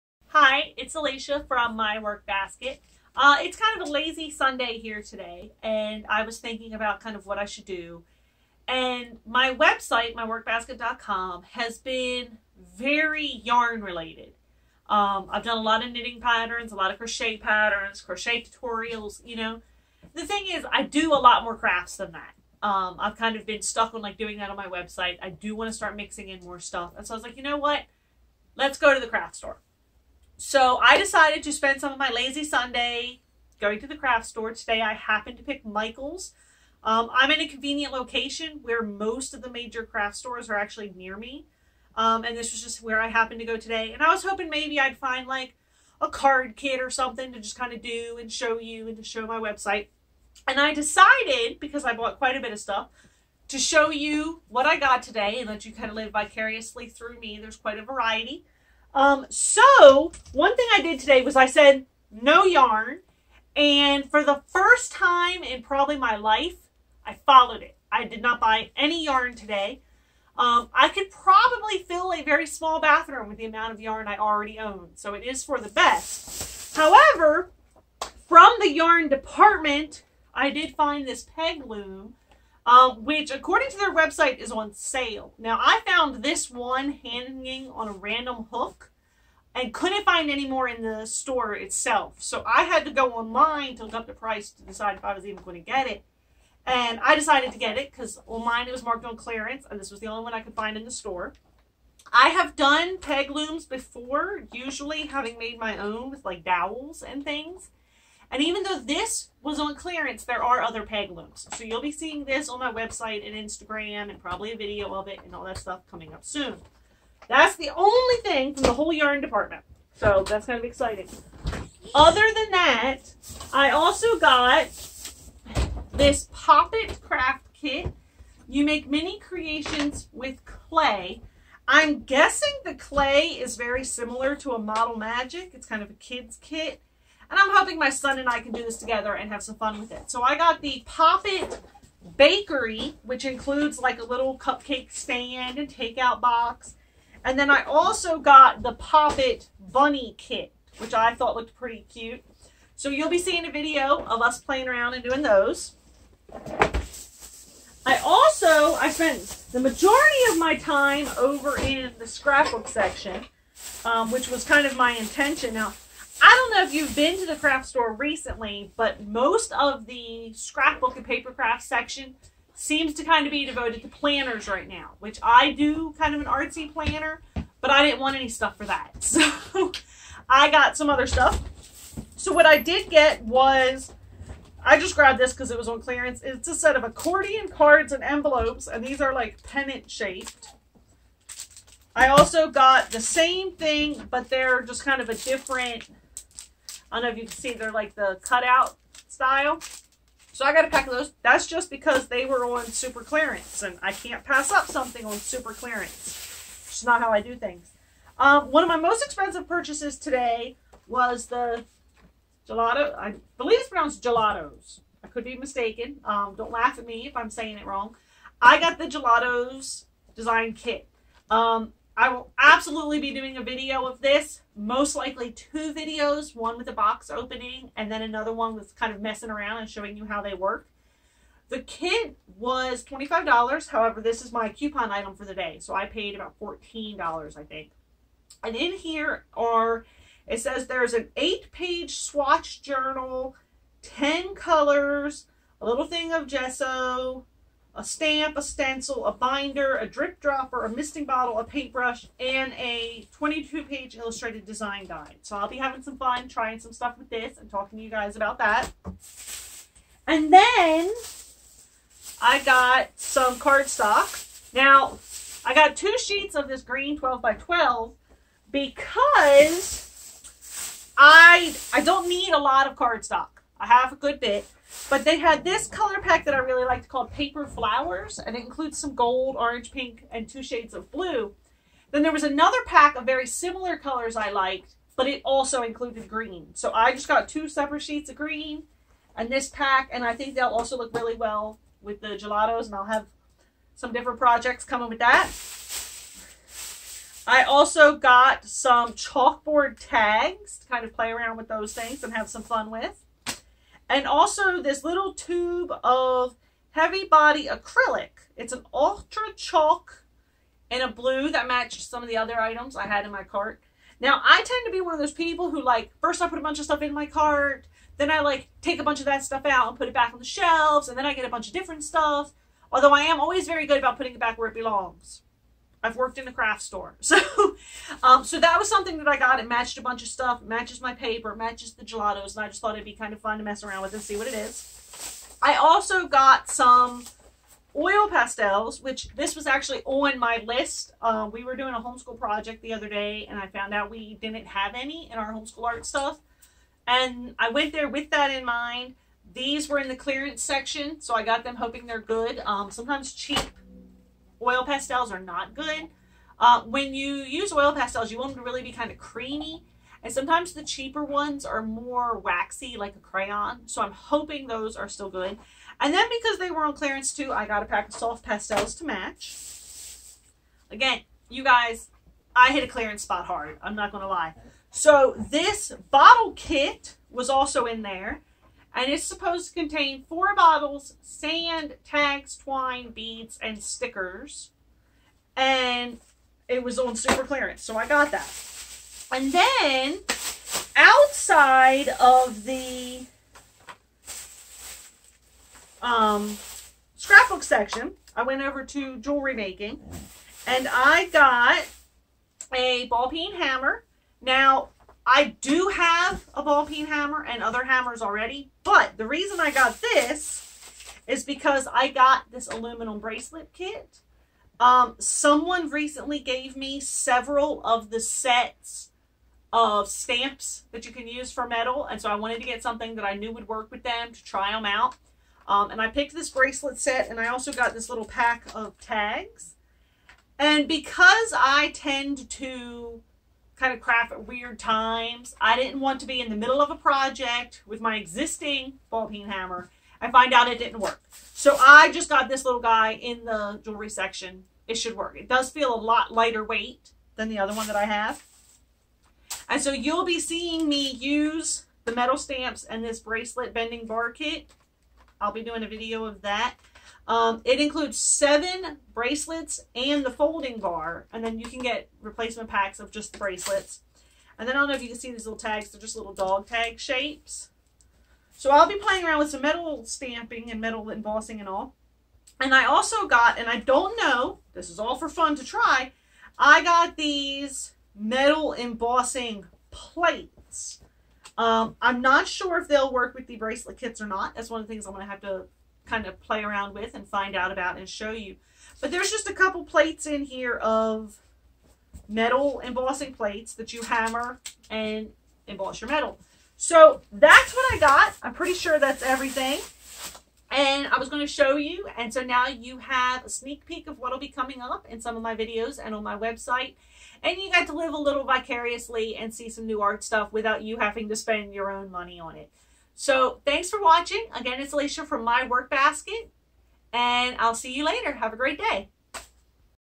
Craft Store Haul
Decided to spend some of my time today leisurely walking the craft store and snagging some new supplies. Here's a video to show my haul and give a sneak peek of what will be popping up on my website and here on youtube!
Be sure to visit me at MyWorkbasket.com